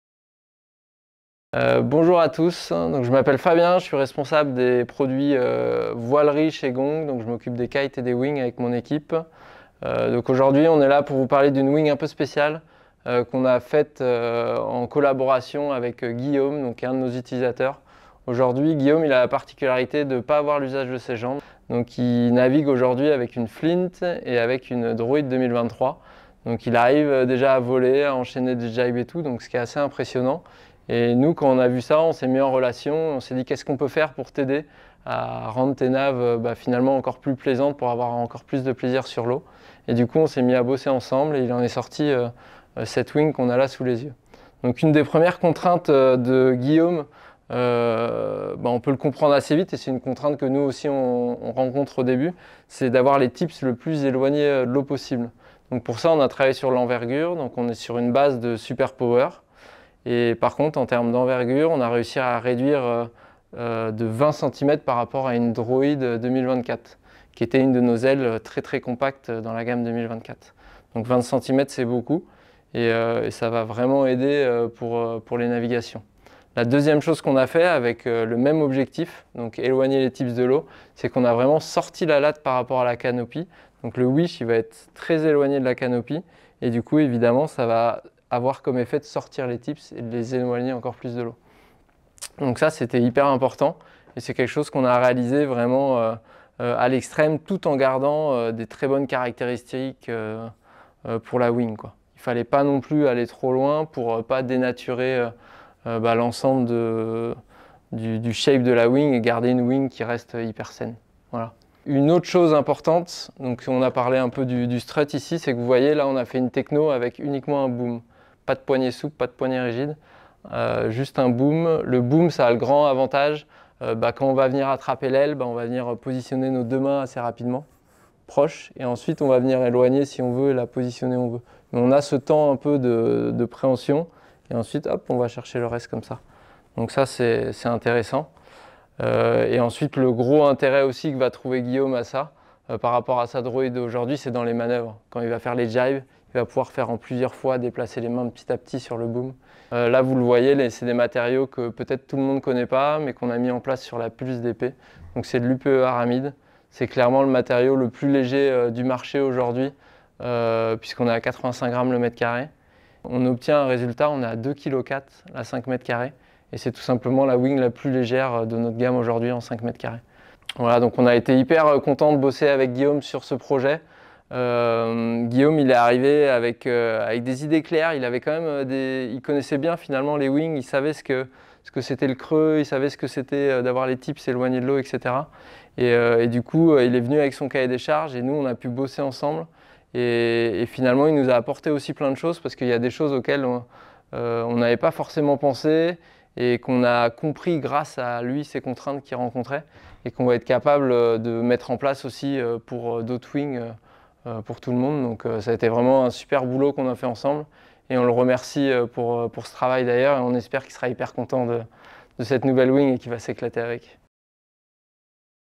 TECH TALK: SUPERPOWER CUSTOM
A wing specially developed for Guillaume Colin is presented to you by our product manager.

Several weeks ago, Guillaume made heads turn after sharing his first wing foil flights on social media despite being paralyzed from the waist down. In doing so, he achieved two superb demonstrations. First, he showed what a passionate person can accomplish to reconnect with the sensations that had him hooked on boardsports. And secondly, the accessibility of wing foiling to people with disabilities with good supervision, motivation and adapted equipment. It is to work on this last point that a collaboration was born between Guillaume and the research and development team of GONG. This 5m2 Superpower Custom, which Fabien presents to you in this video, is the first result of this work.